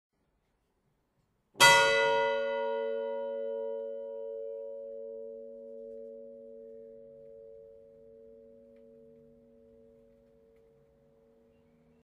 Cloche Cagny (14) tintement C2 - HD
Ensemble campanaire de 2 cloches sonnant sol#3 et la#3, fondues par François Bailly IV en Normandie en 1818.